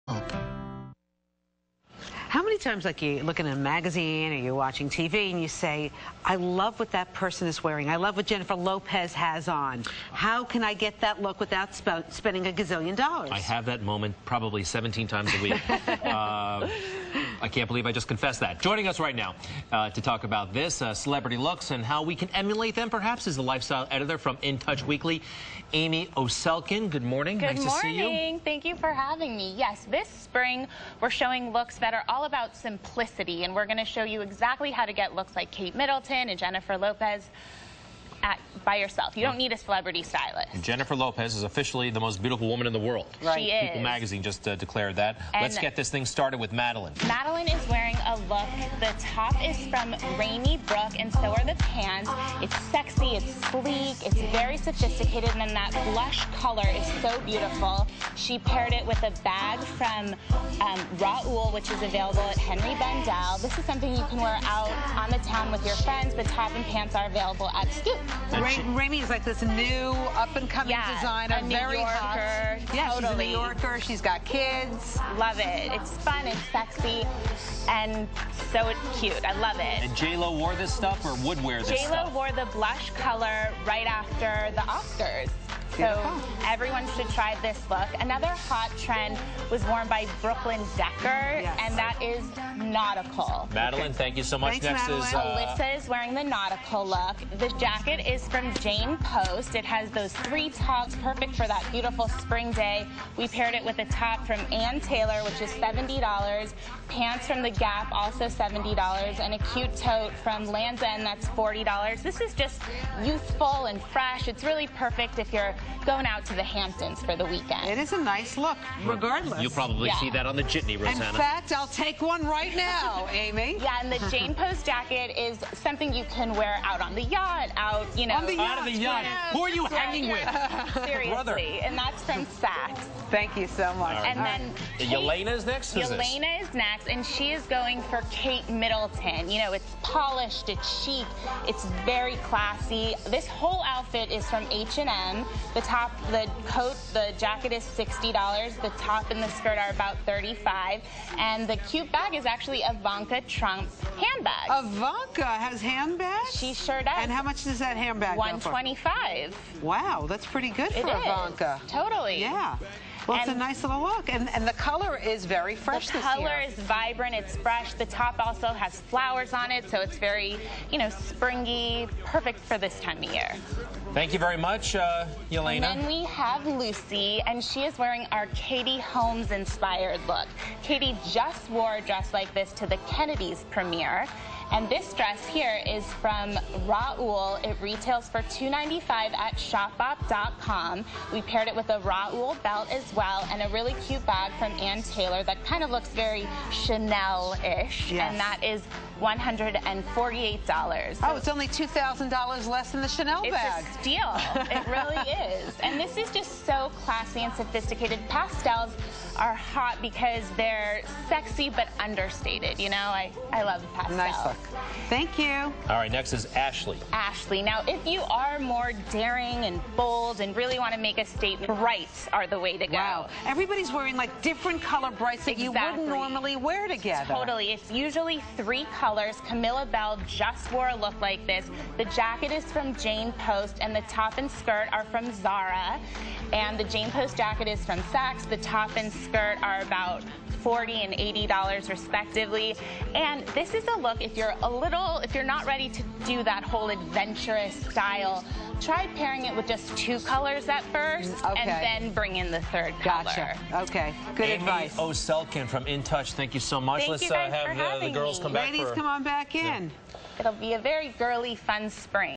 Spring 2011 Trends
Amy Oselkin from In Touch Weekly shows the latest spring 2011 fashion trends